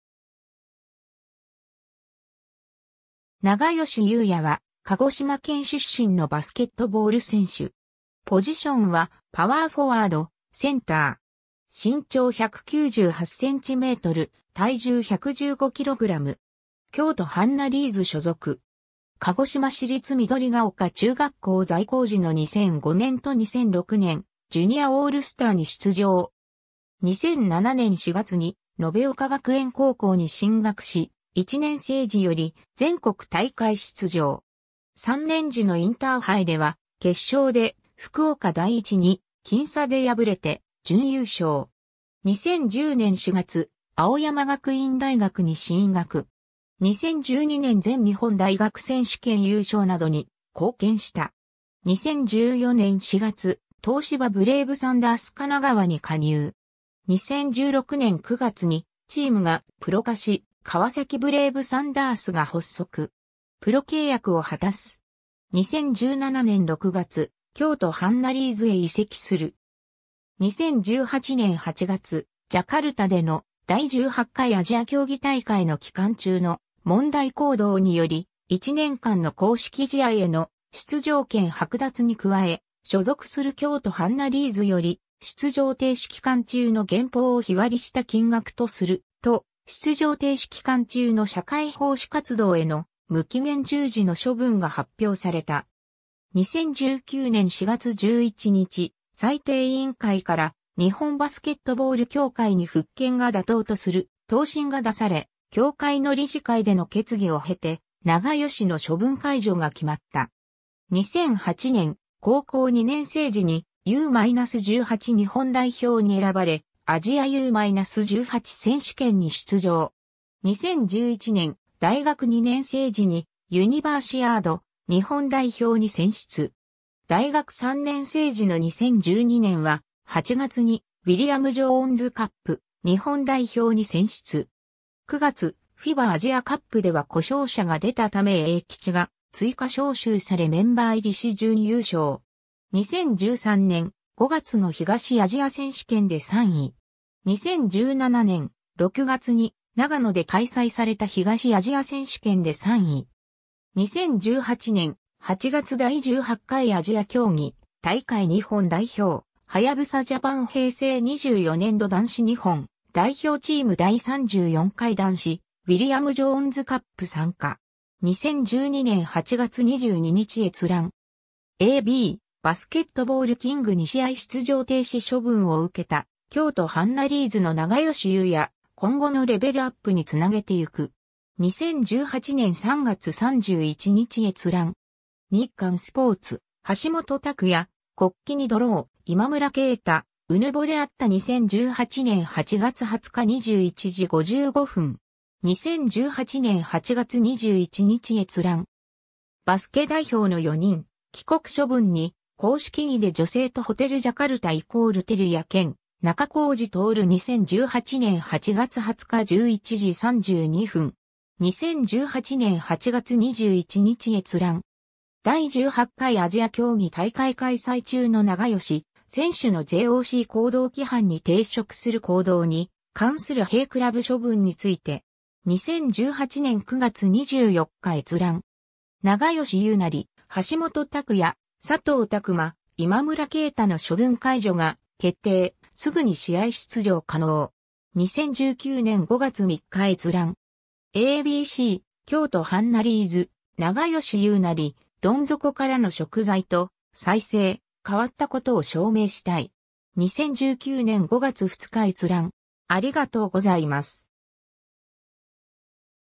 永吉佑也
永吉 佑也（ながよし ゆうや、1991年7月14日 - ）は、鹿児島県出身のバスケットボール選手。
ポジションはパワーフォワード/センター。
京都ハンナリーズ所属。
 鹿児島市立緑丘中学校在校時の2005年と2006年、ジュニアオールスターに出場。
 2007年4月に延岡学園高校に進学し、1年生時より全国大会出場。
3年次のインターハイでは決勝で福岡第一に僅差で敗れて準優勝。
 2010年4月、青山学院大学に進学。
2012年全日本大学選手権優勝などに貢献した。
 2014年4月、東芝ブレイブサンダース神奈川に加入。
 2016年9月にチームがプロ化し、「川崎ブレイブサンダース」が発足。
プロ契約を果たす。
 2017年6月、京都ハンナリーズへ移籍する。
 2018年8月、ジャカルタでの第18回アジア競技大会の期間中の問題行動により、１年間（2018年8月29日～2019年8月28日）の公式試合への出場権はく奪に加え、所属する京都ハンナリーズより、出場停止期間中の減俸（出場停止期間中の報酬は、B1リーグ最低年俸（300万円）を日割りした金額とする）と、出場停止期間中の社会奉仕活動への無期限従事の処分が発表された。
 2019年4月11日、裁定委員会から日本バスケットボール協会に復権が妥当とする答申が出され、協会の理事会での決議を経て、永吉の処分解除が決まった。
 2008年、高校2年生時にU-18日本代表に選ばれ、アジアU-18選手権に出場。
2011年、大学2年生時にユニバーシアード日本代表に選出。
 大学3年生時の2012年は、8月にウィリアム・ジョーンズカップ日本代表に選出。
9月、FIBAアジアカップでは故障者が出たため永吉が追加招集されメンバー入りし準優勝。
 2013年、5月の東アジア選手権で3位。
 2017年、6月に長野で開催された東アジア選手権で3位。
 2018年、8月　第18回アジア競技大会 日本代表 ^ “ハヤブサジャパン 平成24年度男子日本代表チーム　第34回男子ウィリアム・ジョーンズカップ参加”. 2012年8月22日閲覧。
 ^ a b “バスケットボールキング 2試合出場停止処分を受けた京都ハンナリーズの永吉佑也「今後のレベルアップにつなげていく」”. 2018年3月31日閲覧。
 ^ “日刊スポーツ - 橋本拓哉「国旗に泥を」今村佳太「うぬぼれあった」2018年8月20日21時55分”. 2018年8月21日閲覧。
 ^ “バスケ代表の４人、帰国処分に　公式着で女性とホテルジャカルタ＝照屋健、中小路徹 2018年8月20日11時32分”. 2018年8月21日閲覧。
 ^ “第18回アジア競技大会開催中の永吉選手のJOC行動規範に抵触する行動に関する弊クラブ処分について”. 2018年9月24日閲覧。
 ^ “永吉佑也、橋本拓哉、佐藤卓磨、今村佳太の処分解除が決定、すぐに試合出場可能”. 2019年5月3日閲覧。
 ^ a b c “京都ハンナリーズ永吉佑也、どん底からの贖罪と再生「変わったことを証明したい」”. 2019年5月2日閲覧。